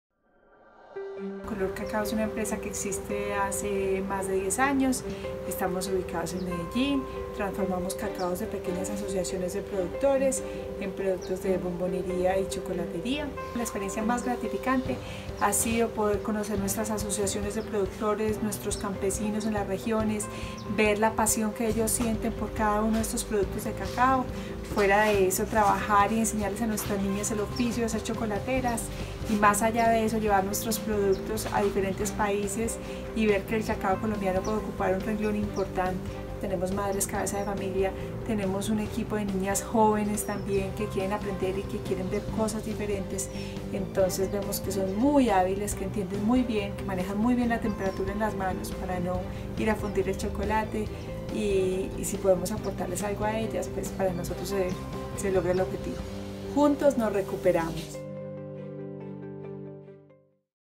¡Conoce más sobre Color Cacao🍫!
En este video les mostramos un poco más sobre lo que hacemos en Color Cacao🍫.

¡Que alegria es saber que contamos con ustedes para seguir trabajando en lo que amamos🥰!

Seguiremos trabajando con campesinos colombianos y mujeres colombianas amantes del chocolate 😍